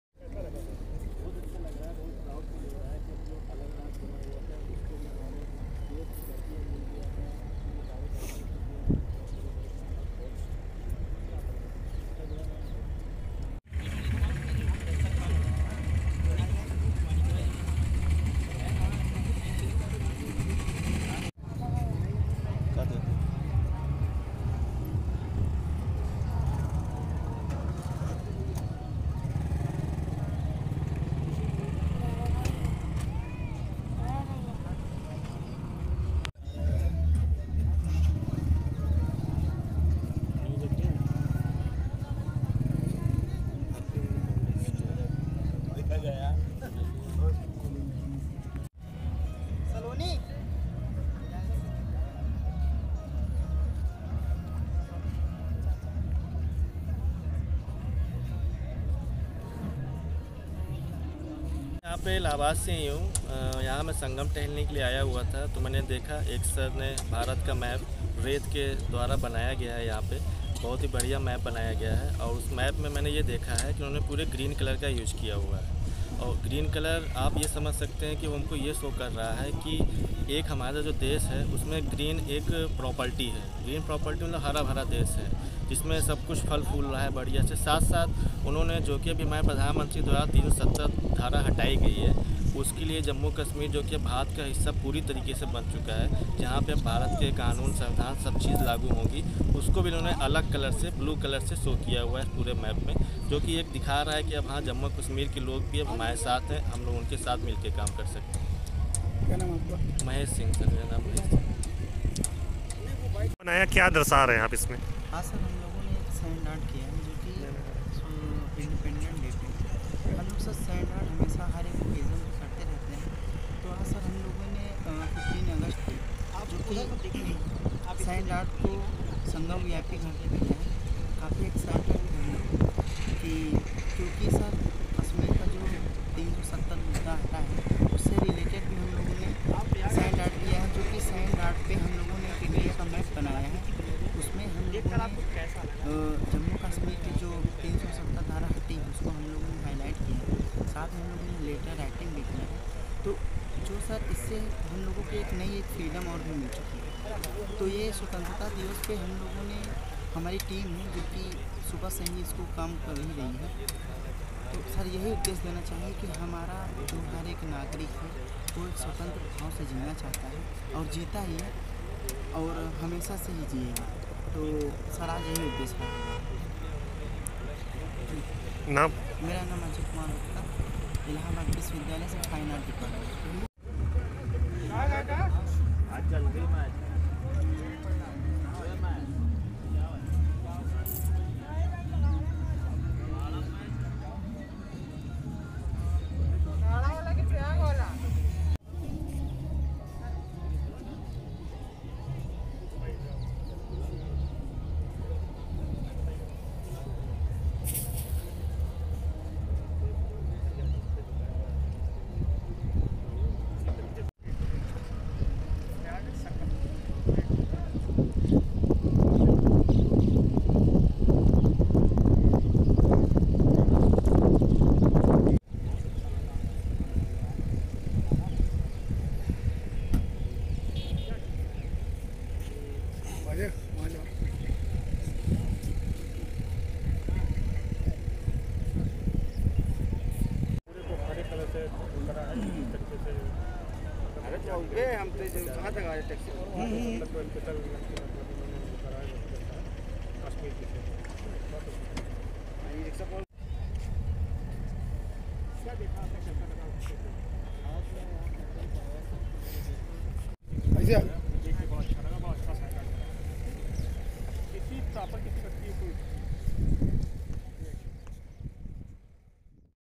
प्रयागराज में जश्न-ए-आजादी की धूम, तिरंगे में रंगा देश
पूरे देश में आजादी का जश्न मनाया जा रहा है। संगम नगरी प्रयागराज में भी आजादी के जश्न की धूम है। पूरे देश में लोग अलग-अलग तरीके से जश्न मना रहे हैं। प्रयागराज में सैंड आर्ट कलाकार ने संगम की रेती पर इंडिपेंडेंस डे की तस्वीरें उकेरी हैं और अपने तरीके से लोगों को जश्न-ए-आजादी की बधाई दी। उधर, बजरंग दल से जुड़े हुए कार्यकर्ताओं ने अखंड भारत दिवस मनाया। इस मौके पर अखंड भारत के नक्शे पर मोमबत्ती जलाकर यह संदेश दिया गया कि हिंदुस्तान की अखंडता व राष्ट्रीयता से जो कोई खिलवाड़ करेगा, उसका मुंहतोड़ जवाब दिया जाएगा। भारत के 72वें स्वतंत्रता दिवस के पूर्व ज़ोरदार जश्न मना कर भारत के संविधान व इसकी सीमाओं के रक्षा का संकल्प लिया गया। देश के नक्शे को  मोमबत्ती से सजाया गया और लोगों को स्वतंत्रता दिवस की बधाइयां दी गईं।